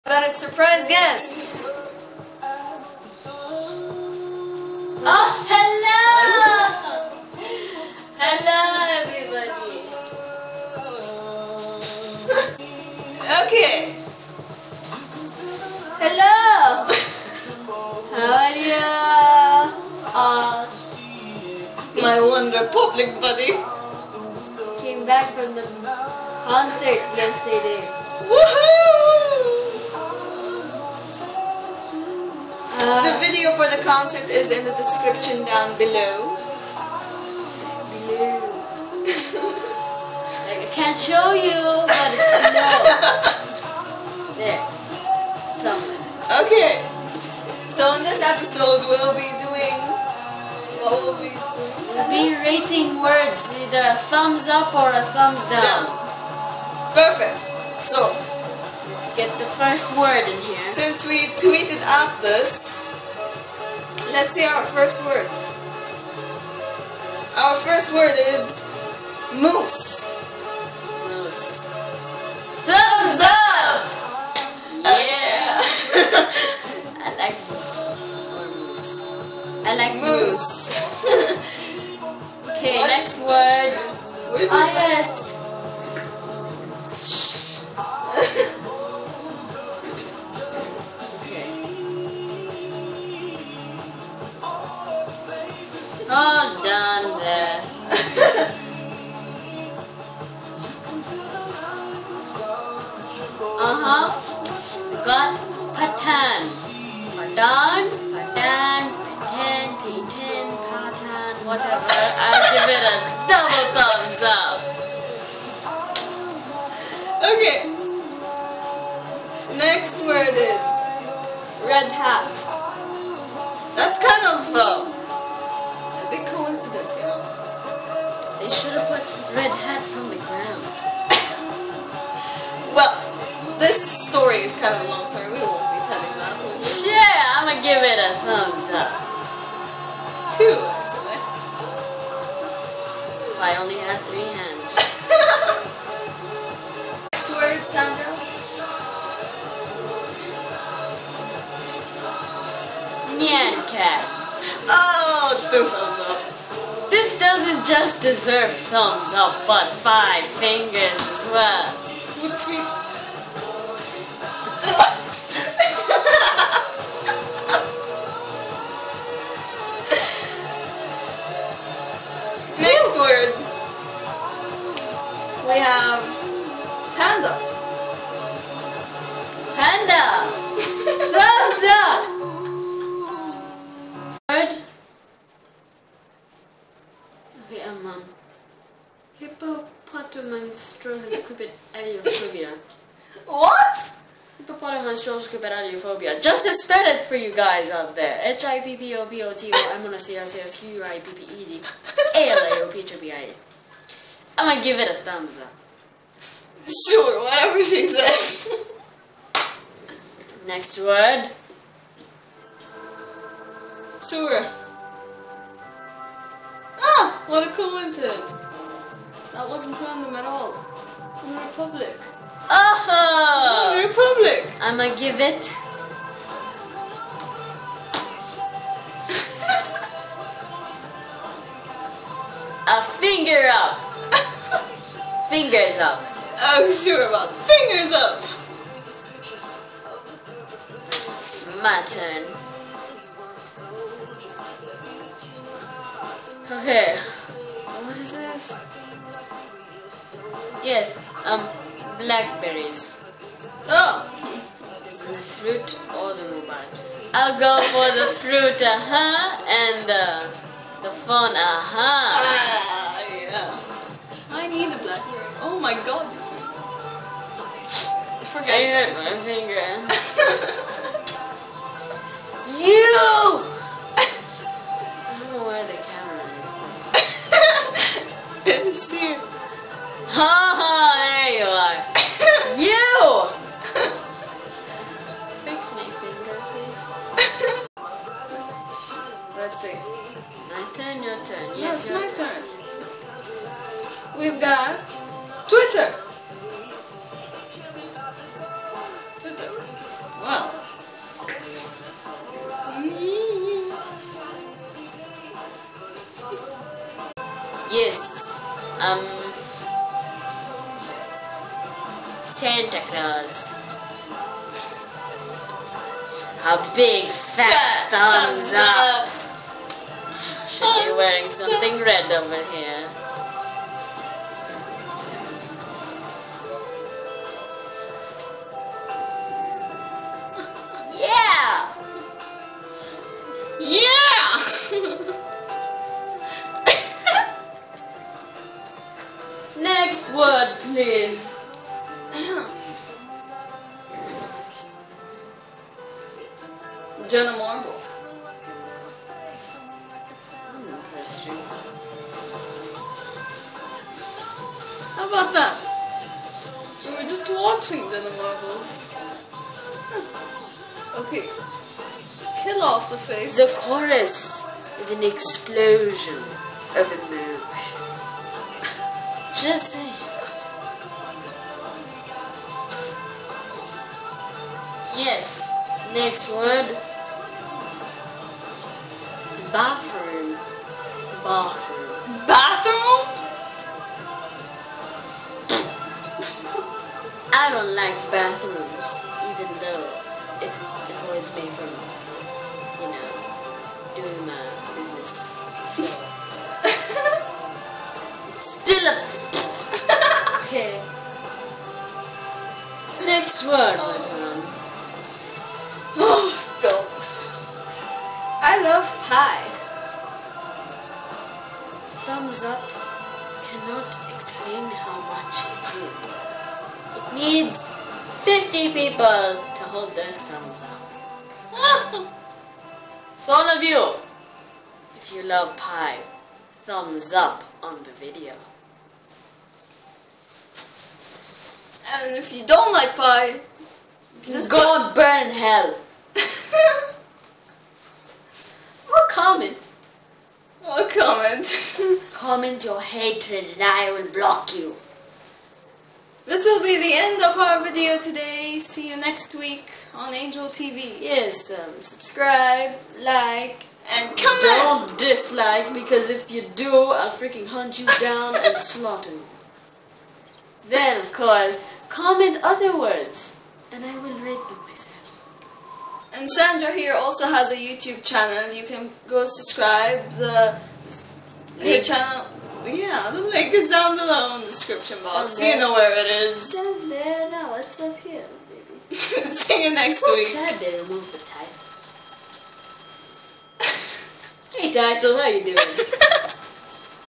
Rate the word
Me and my friend Sandra have taken a bunch of random words from Twitter and rated them